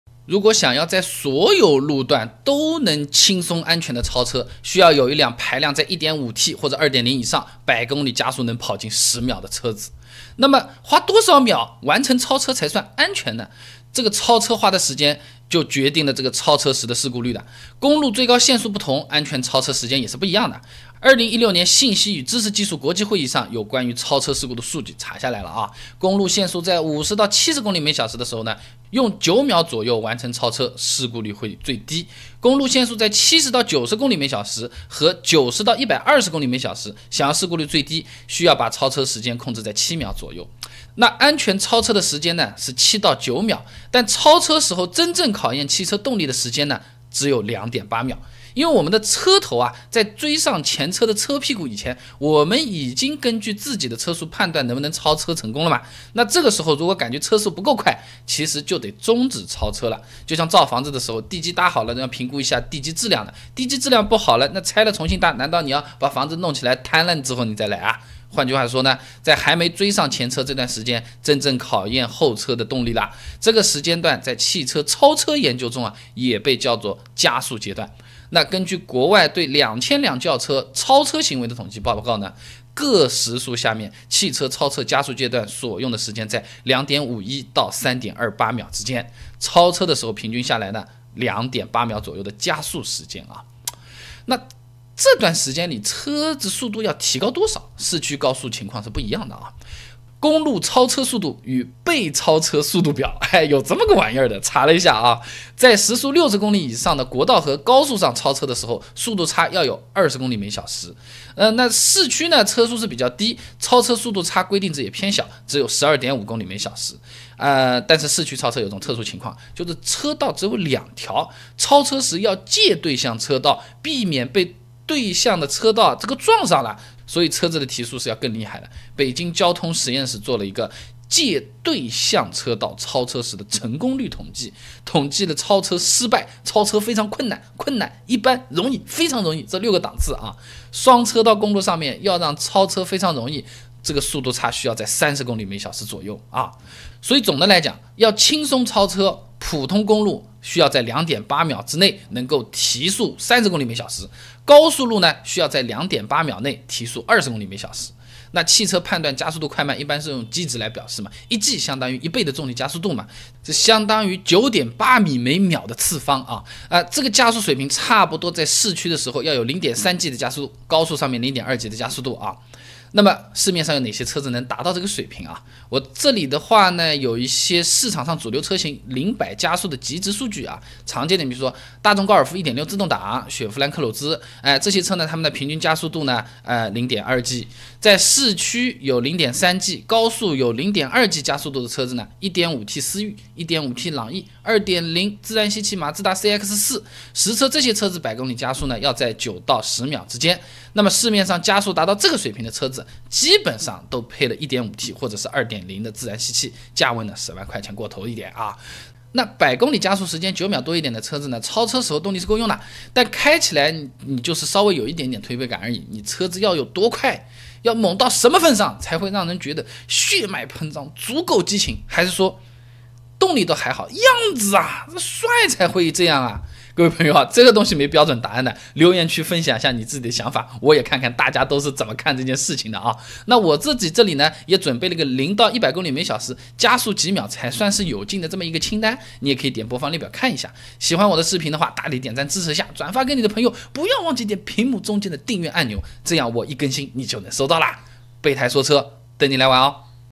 車子有多少動力，超車時才夠用？
多快完成超車才安全？車子動力多少才夠用？